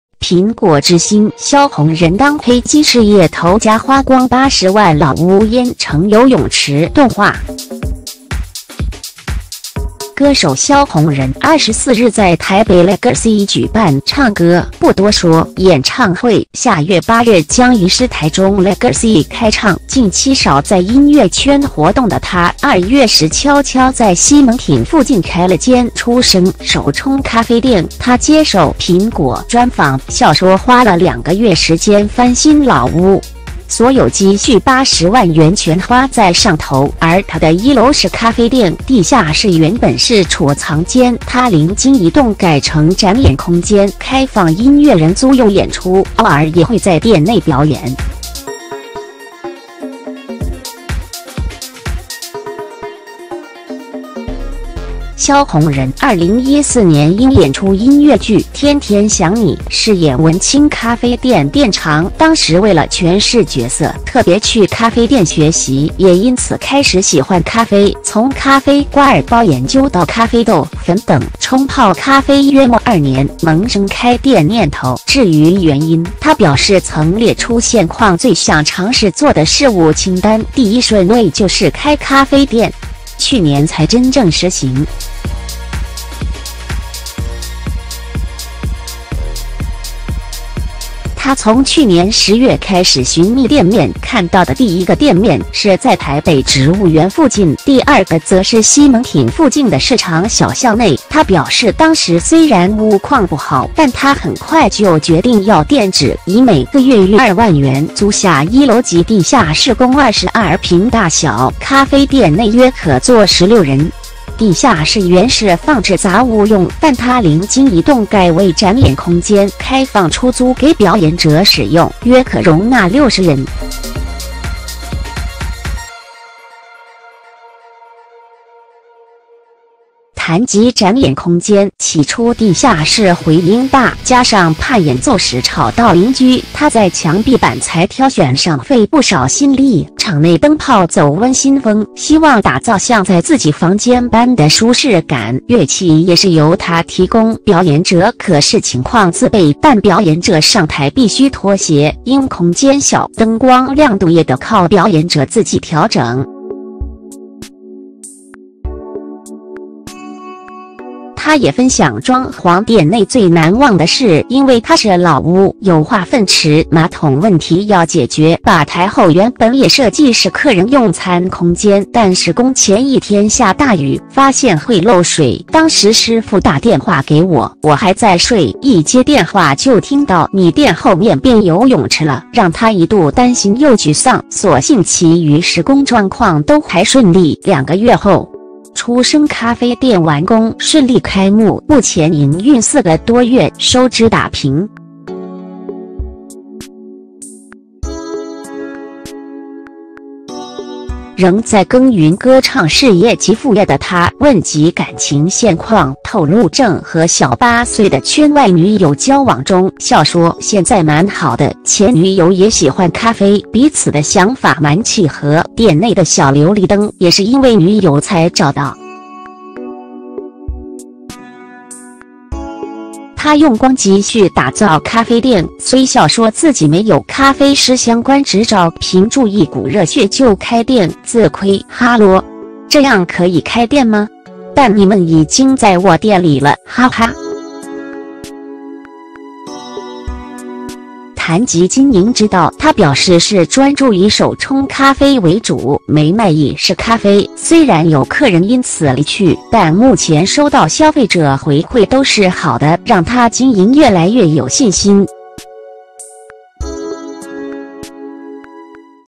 【蘋果之星】蕭閎仁當黑金事業頭家 花光80萬老屋淹成游泳池(動畫)
歌手蕭閎仁24日在台北Legacy舉辦「唱歌，不多說」演唱會，下月8日將移師台中Legacy開唱，近期少在音樂圈活動的他，2月時悄悄在西門町附近開了間「初聲」手沖咖啡店，他接受《蘋果》專訪，笑說花了2個月時間翻新老屋，所有積蓄80萬元全花在上頭，而他的1樓是咖啡店，地下室原本是儲藏間，他靈機一動改成展演空間，開放音樂人租用演出，偶爾也會在店內表演。
蕭閎仁2014年因演出音樂劇《天天想你》，飾演文青咖啡店店長，當時為了詮釋角色，特別去咖啡店學習，也因此開始喜歡咖啡，從咖啡掛耳包研究到咖啡豆、粉等，沖泡咖啡約莫2年，萌生開店念頭，至於原因，他表示曾列出現況最想嘗試做的事物清單，第一順位就是開咖啡店，去年才真正實行。
他從去年10月開始尋覓店面，看到的第一個店面，是在台北植物園附近，第二個則是西門町附近的市場小巷內，他表示當時雖然屋況不好，但他很快就決定要店址，以每個月逾2萬元租下1樓及地下室共22坪大小，咖啡店內約可坐16人，地下室原是放置雜物用，但他靈機一動改為展演空間，開放出租給表演者使用，約可容納60